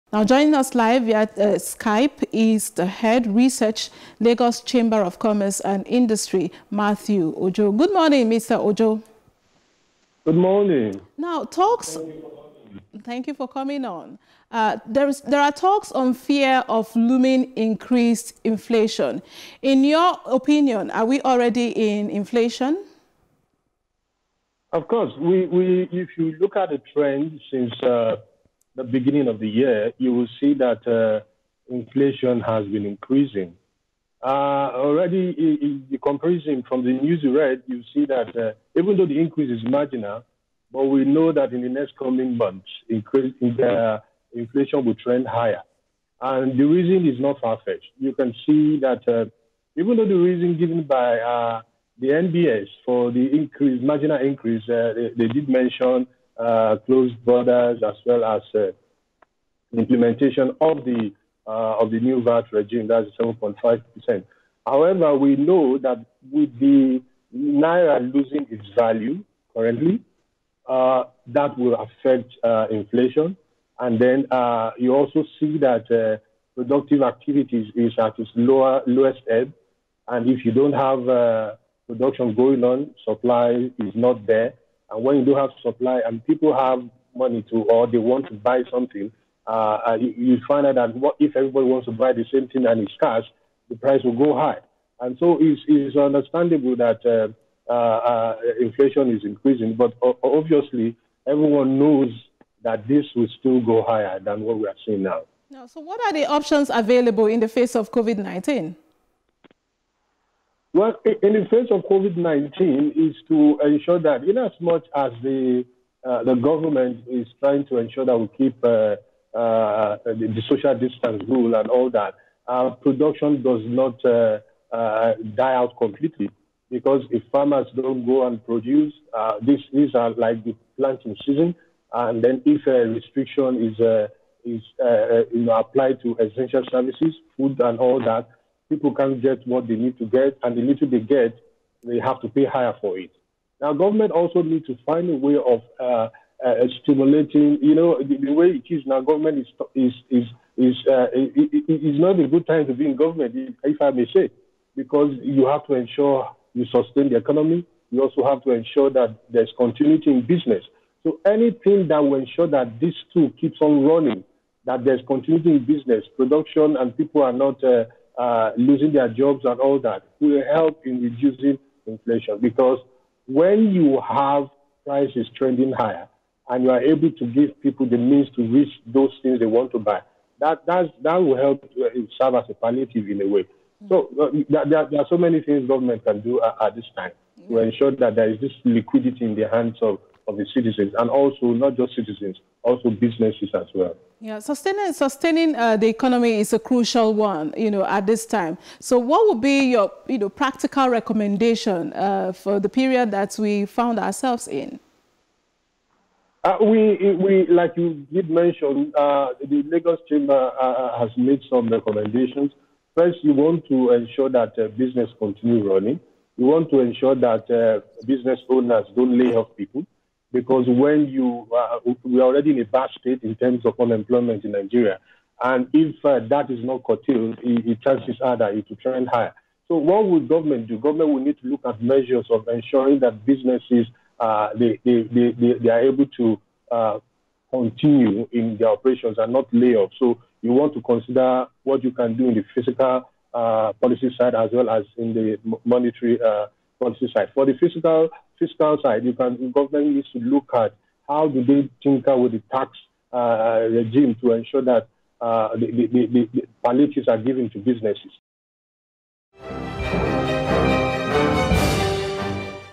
Continuity In Business And Production Will Reduce Inflation - Matthew Ojo
The Lagos Chamber of Commerce and Industry, Nigeria Employers’ Consultative Association, and the Association of Small Business Owners of Nigeria have warned that unless the Federal Government take some measures to reposition the economy, tougher times await Nigerians.

Matthew Ojo, Head Research In LCCI joined the studio via Skype.